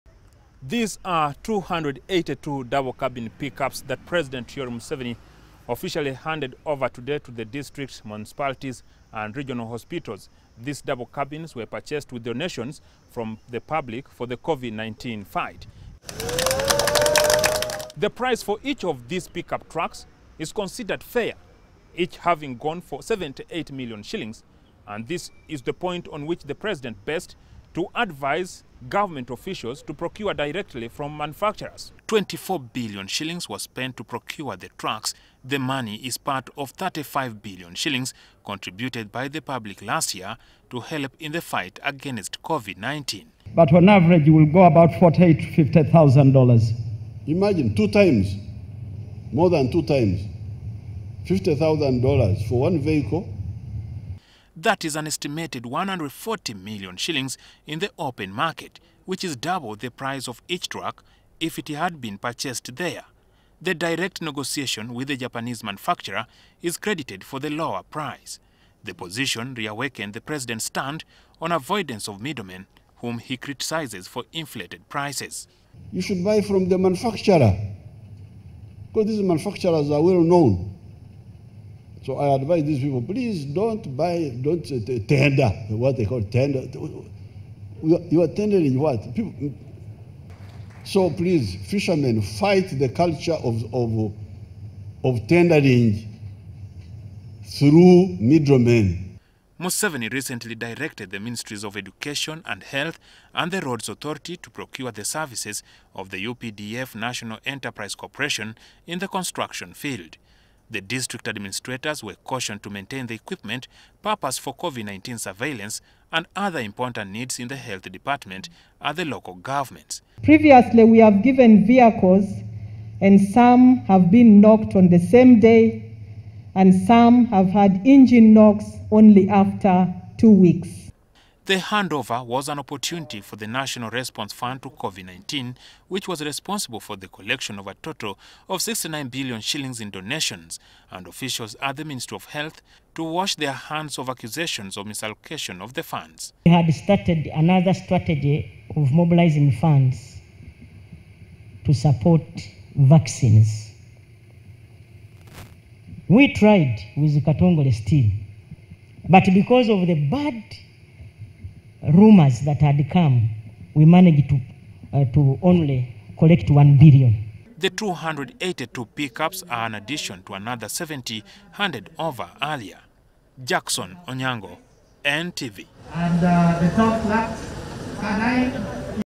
Museveni recommends direct procurement from manufacturers
President Yoweri Museveni has advised government technocrats to stop procuring services through tenders but instead use direct procurement from manufacturers. This stand is premised on the price of more than 200 pickup trucks procured with donations from the public in the fight against COVID-19.